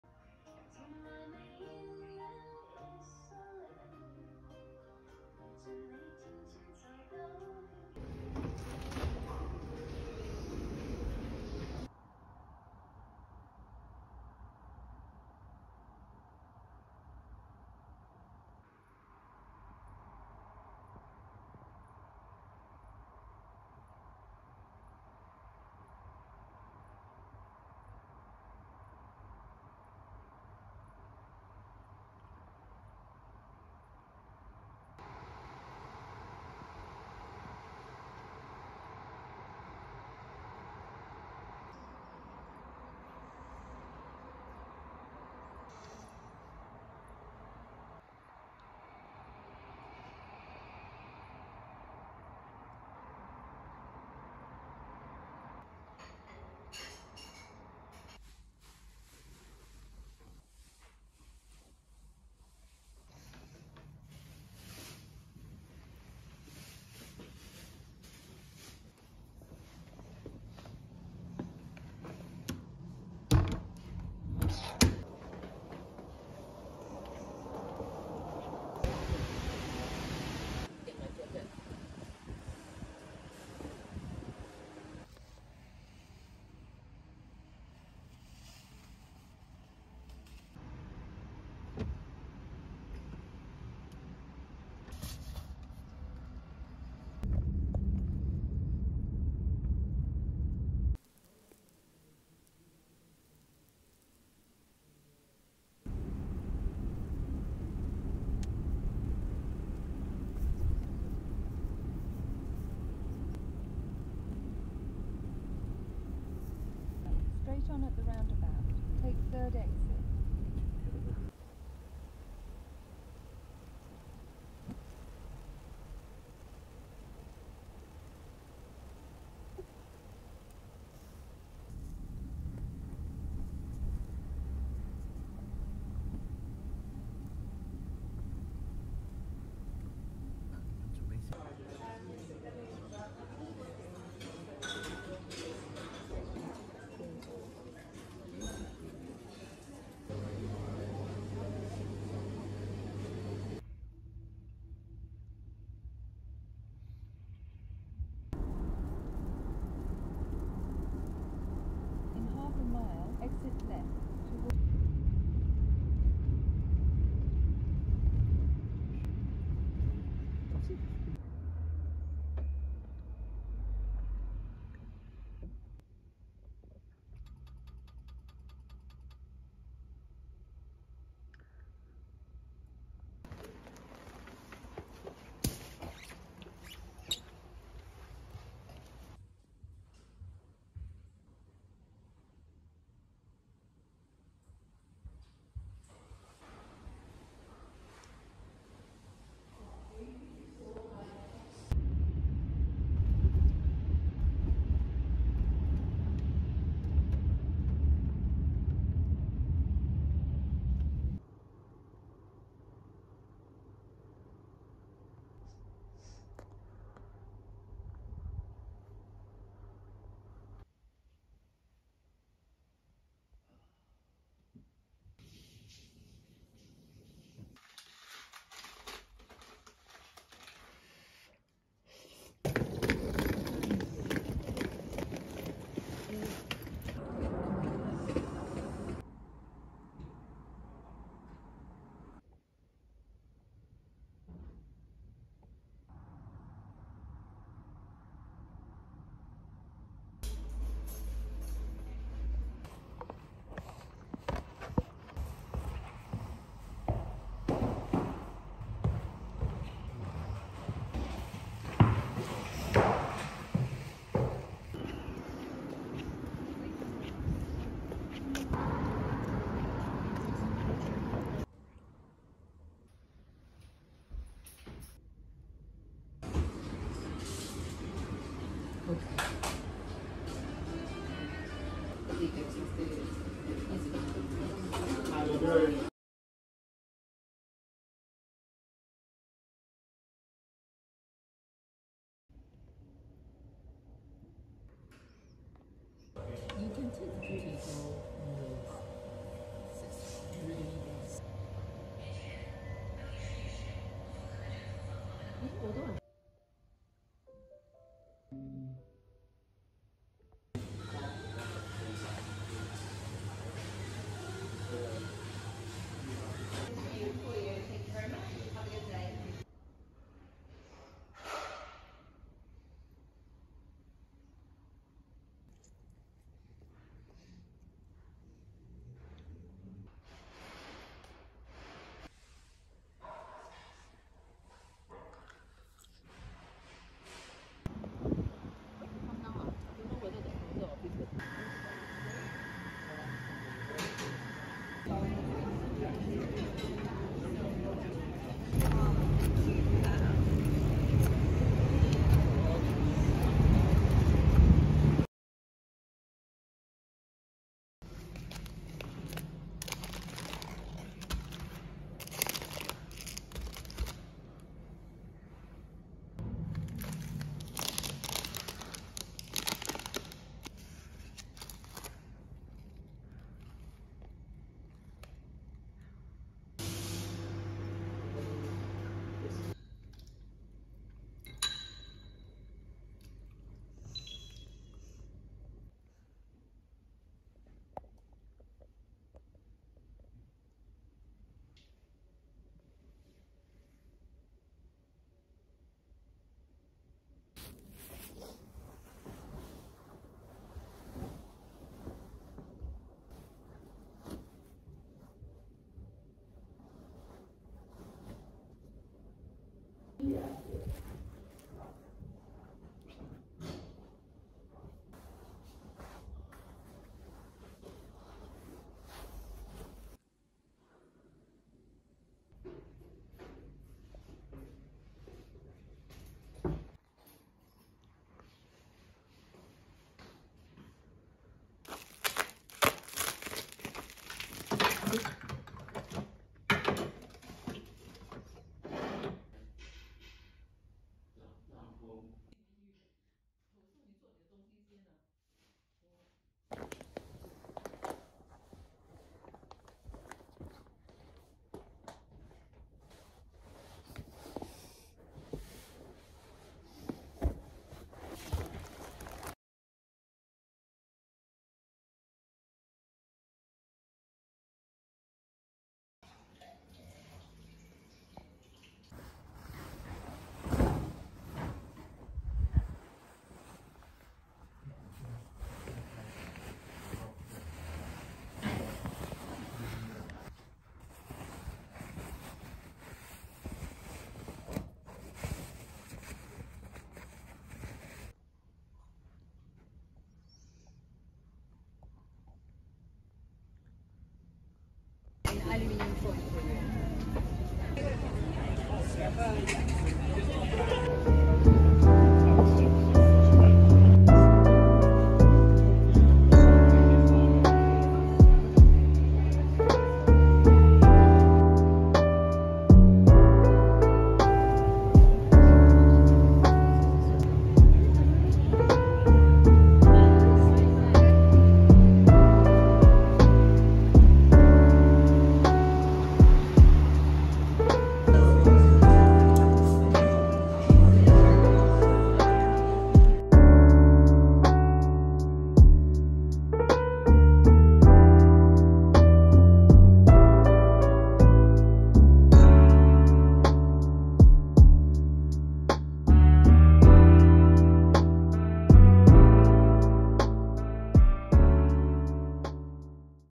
vlog 📦 moving house (again) | unpacking & settling in | street food fair 🥡🥤
↷ ✦ ꒰✎ * 𝐠𝐨𝐥𝐝𝐞𝐧𝐡𝐨𝐮𝐫 𝐢𝐬 𝐭𝐲𝐩𝐢𝐧𝐠 ...
hello! in this video, i finish packing for the move and head out to where i'll be spending the next year doing my postgraduate degree. i used to live in this city when i was a teenager and not much has changed, so it isn't a tough adjustment. i'm excited to start putting together my new room!